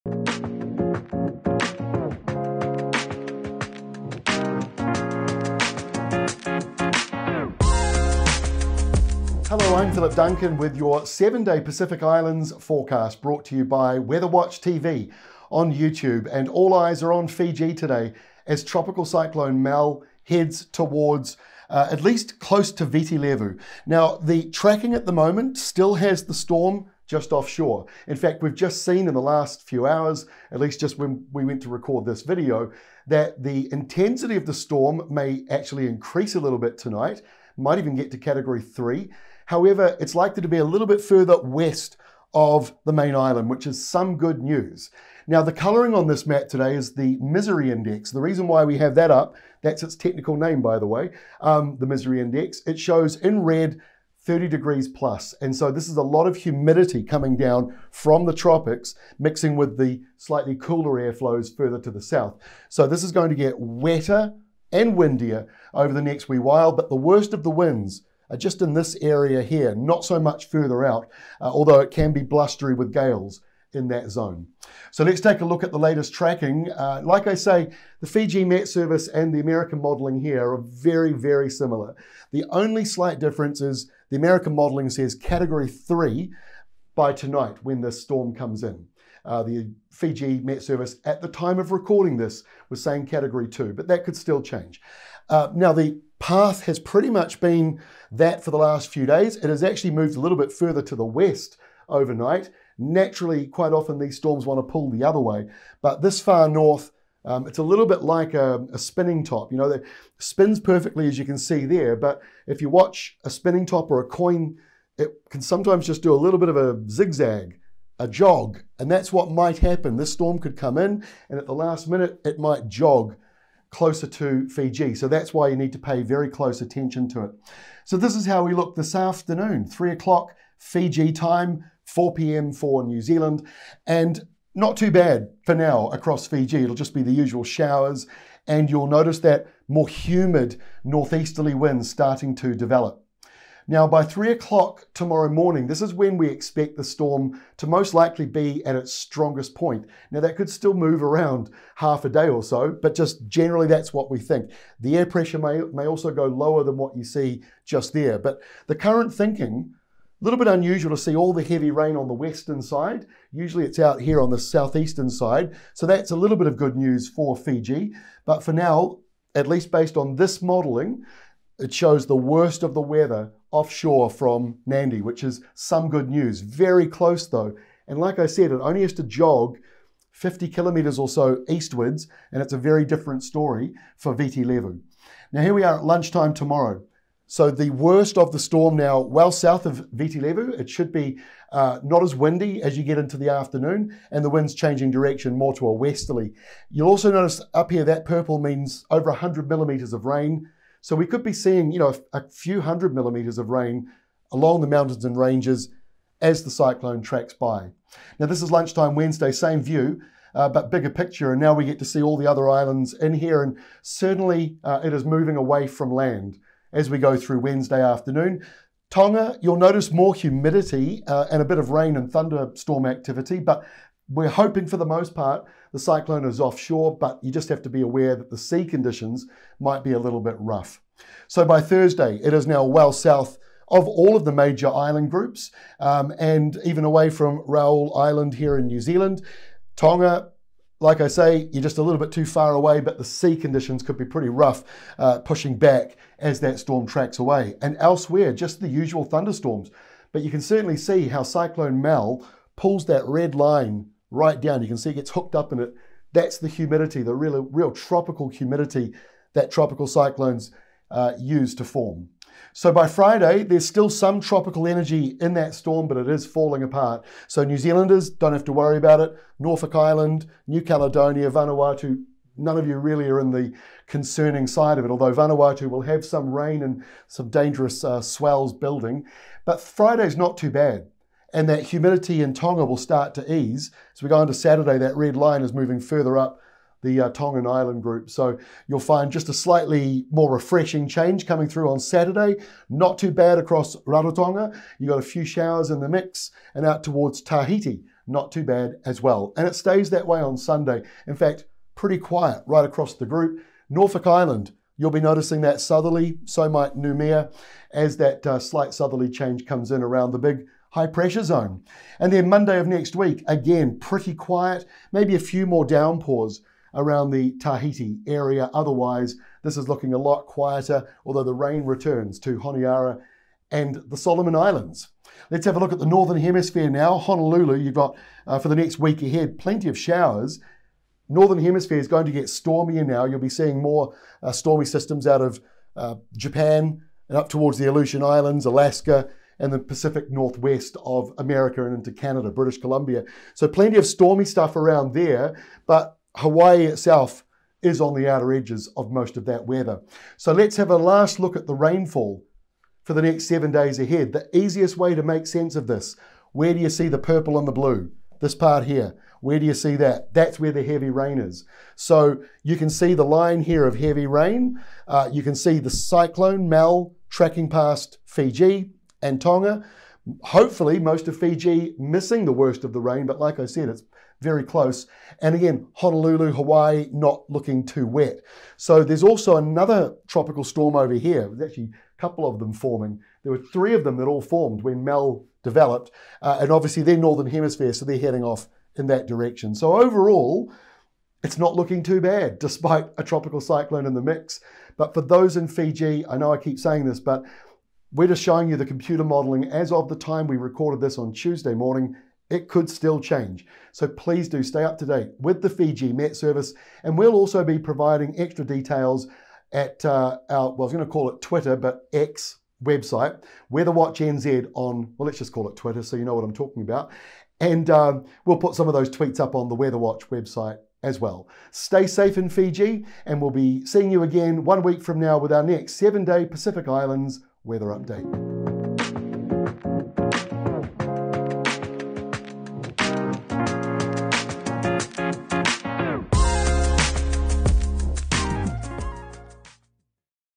Tropical Cyclone Mal heads to FIJI + 7 Day Pacific Islands forecast
A strengthening Tropical Cyclone is heading towards Fiji today and tonight and may even reach Severe Category 3 status.  The latest tracking does show the centre of Mal hopefully tracking further west of Viti Levu. As our video says, it may still make a more direct hit and those in Fiji should pay very close attention to the Fiji Met Service and other weather providers across Tuesday and Wednesday.

The storm exits Fiji on Wednesday and may bring extra humidity and dangerous seas to around Tonga. Otherwise Mal should fall apart north of NZ.

Elsewhere and the tropics aren’t too active or stormy - but the Northern Hemisphere is showing signs of more life than previous months.

And we have a 7 Day Rainfall Map for the entire Pacific region - to help work out where the heaviest rain and cyclones will track.